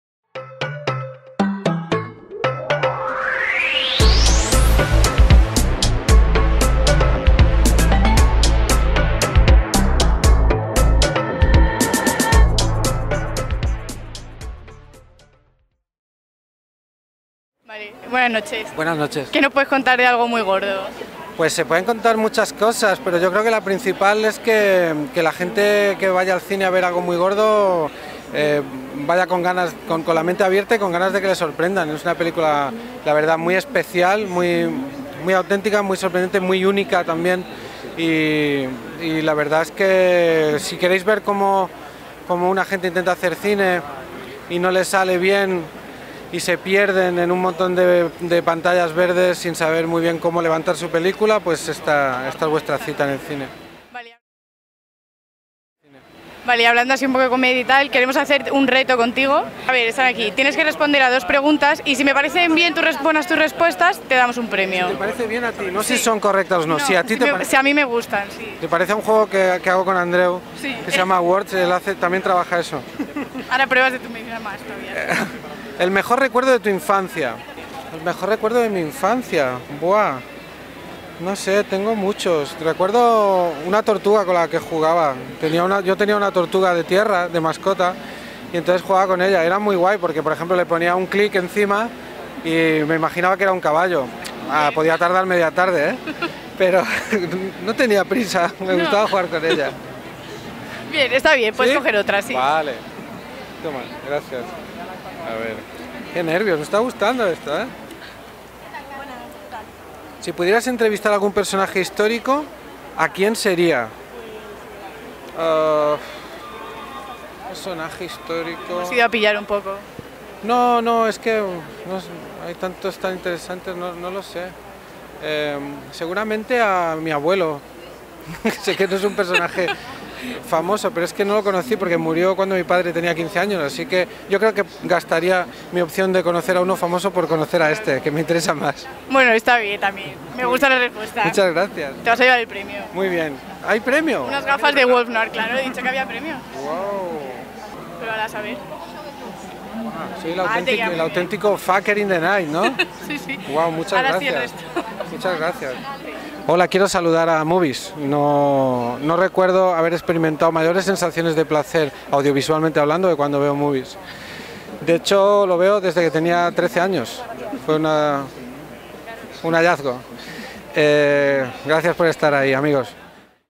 Entrevista | Berto Romero y el Reto Wolfnoir
Berto Romero estrena en cines Algo muy gordo y pasó nuestro